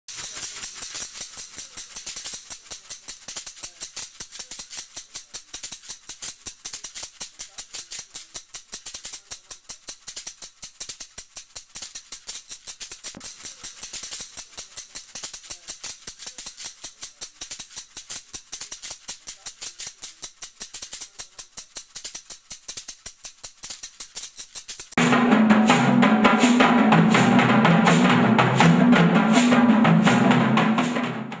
Oyo Rattle
This demostration acknowledges the significance of the Oyo Rattle in Iboland, Nigeria.

For more information about us and the aspects of Nigerian culture, please